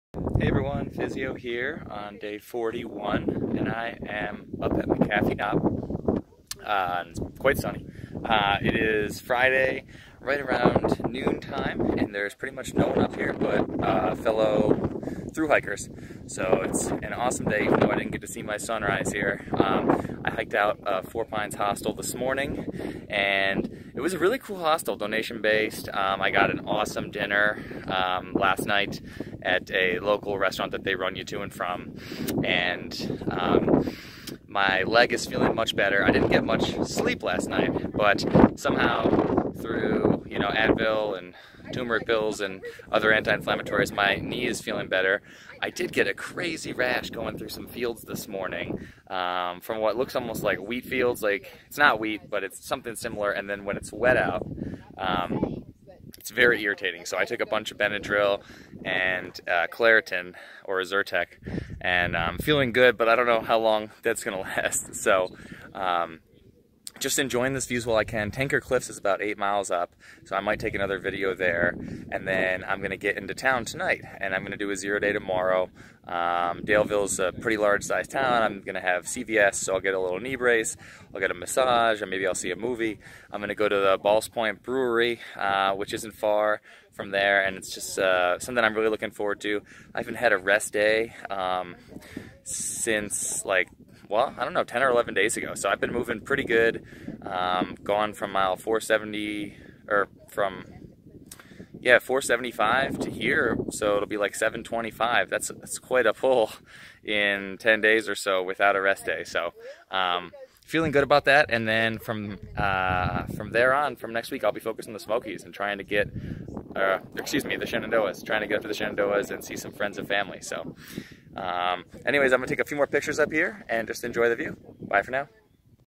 Appalachian Trail Day 41 McAfee Knob
It's Day 41 and I am up at Mcafee Knob. It's quite sunny, it's Friday right around noon time, and there's pretty much no one up here but fellow thru-hikers. So it's an awesome day even though I didn't get to see my sunrise here. I hiked out of Four Pines Hostel this morning which was a really cool hostel (donation-based).  I got an awesome dinner last night at a local restaurant that they drive you to and from and my leg is feeling much better. I didn't get much sleep last night, but somehow through Advil and turmeric pills and other anti-inflammatories - my knee is feeling better. I did get a crazy rash going through some fields this morning from what looks almost like wheat.  It's definitely not wheat but it's something similar and then when it's wet out it's very irritating.  So I took a bunch of Benadryl and Claritin / Zyrtec and I'm feeling good but I don't know how long that's going to last.

Tinker Cliffs is about another eight miles up and then I'm going to get into town tonight and I'm going to do a zero day tomorrow.  Daleville's a pretty large sized town so I'm going to get a kneebrace at CVS and I'll get a massage and maybe I'll see a movie. I'm going to go to the Ballast Point Brewery, which isn't far from town, and I'm really looking forward to that. I haven't had a rest day since about 10 or 11 days ago so I've been moving pretty good from mile 470/475 to here (~725).  That's quite a bit for 10 days or so without a rest day.  So I'm feeling good about that achievement and next I'll be focused on the the Shenandoahs and seeing some friends and family.

If you're interested in learning more about what I'm doing thru-hiking the Appalachian Trail, head